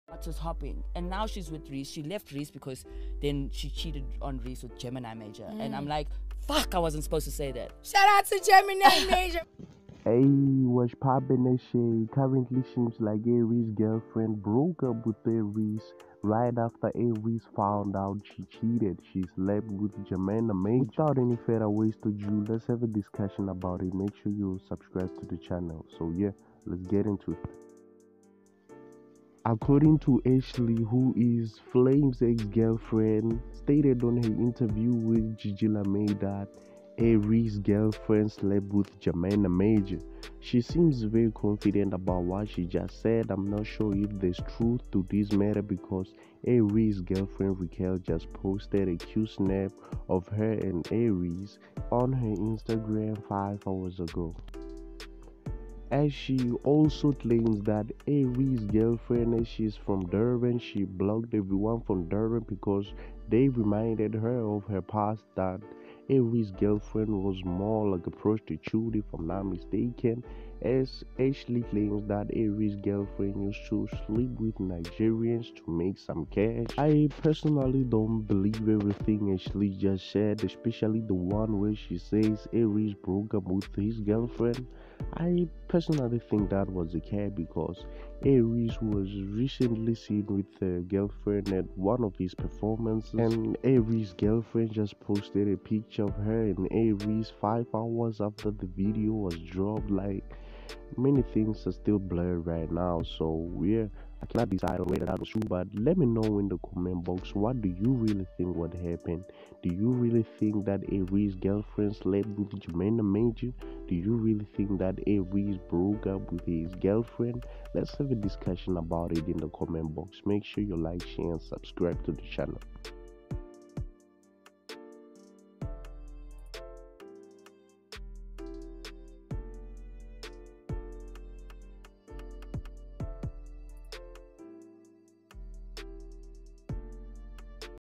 Rickelle Jones Cheats On A-Reece !?
she slept with Gemini major